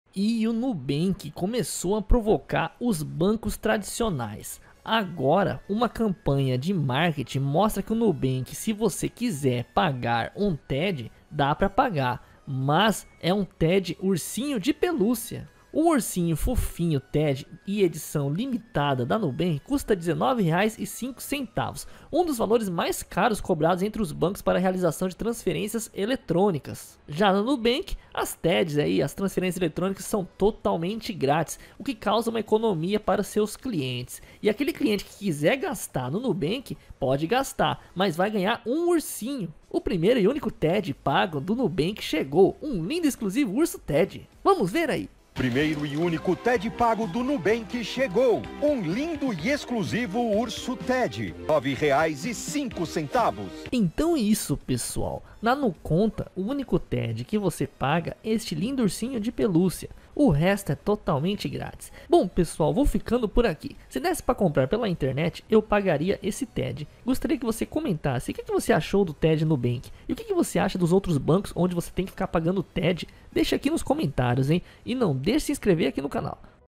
NUBANK TEM COBRANÇA DE URSO TED
Nubank cutuca bancos tradicionais vendendo urso pelo valor da taxa de TED cobrada por eles